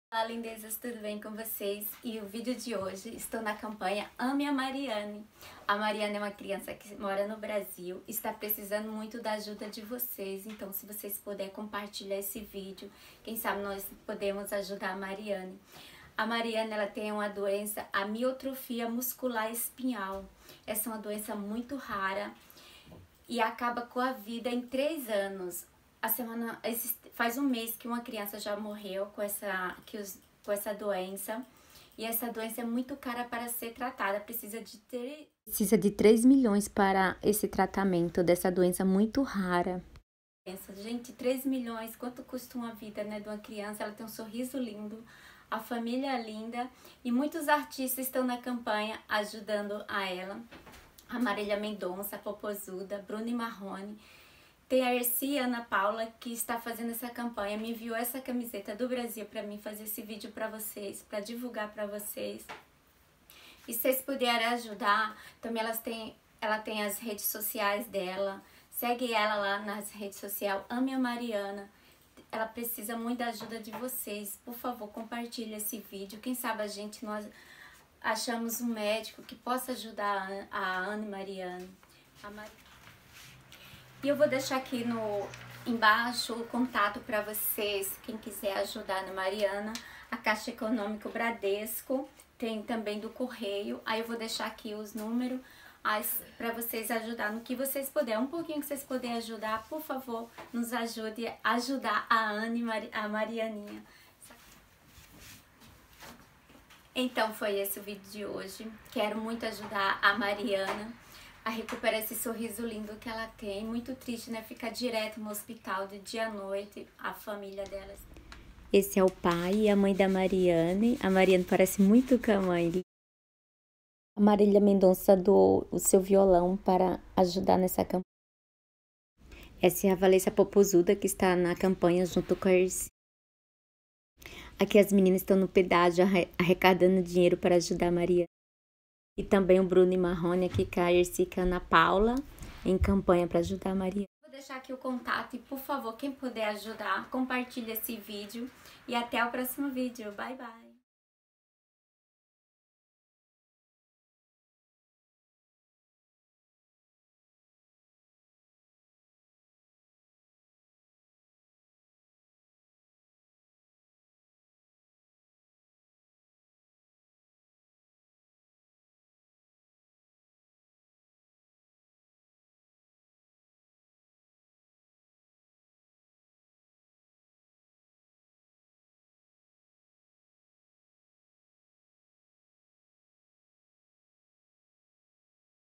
DOE ESPERANÇA, E RECEBERÁ GRATIDÃO DE DEUS
OLÁ LINDEZAS TUDO  BEM  COM  VCS O VIDEO  DE  HOJE  É  UMA  CAMPANHA PARA   AJUDA  MARIANNY . ELA  TEM UMA  DOENÇA  MUITO  RARA  E    É  MUITO  CARO  O  TRATAMENTO .